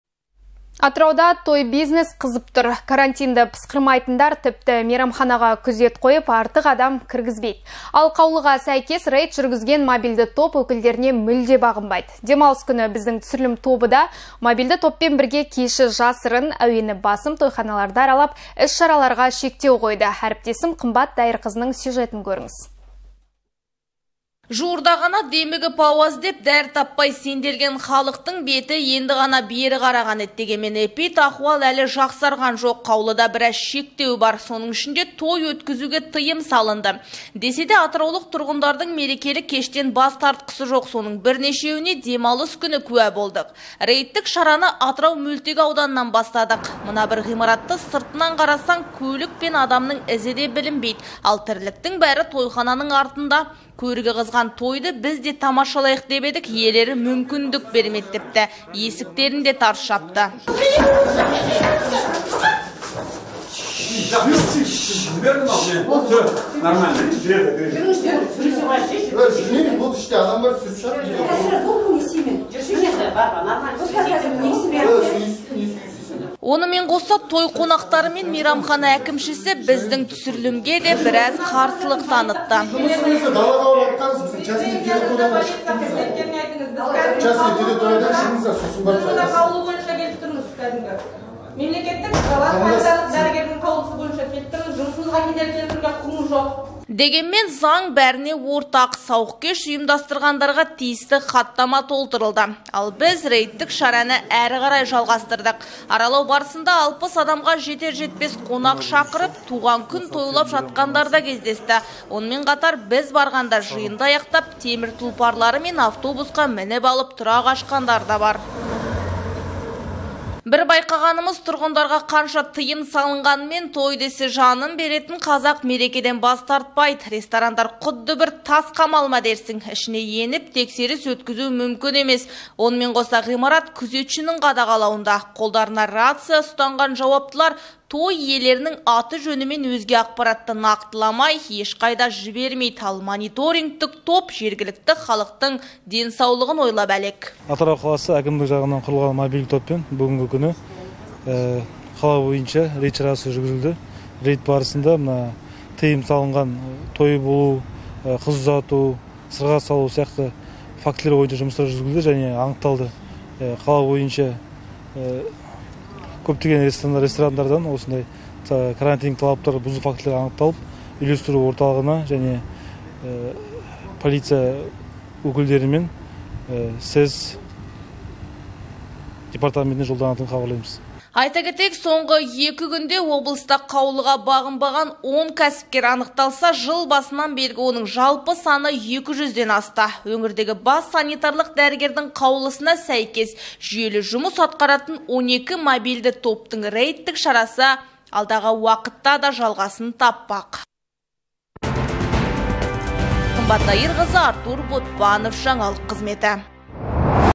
Той-бизнес қызып тұр
Атырауда той-бизнес қызып тұр. Карантинді пысқырмайтындар тіпті мейрамханаға күзет қойып, артық адам кіргізбейді. Ал қаулыға сәйкес рейд жүргізген мобильді топ өкілдеріне мүлде бағынбайды. Демалыс күні біздің түсірілім тобы да мобильді топпен бірге кеші жасырын, әуені басым тойханаларды аралап, іс-шараларға шектеу қойды. Әріптесім Қымбат Даирқызының сюжетін көріңіз.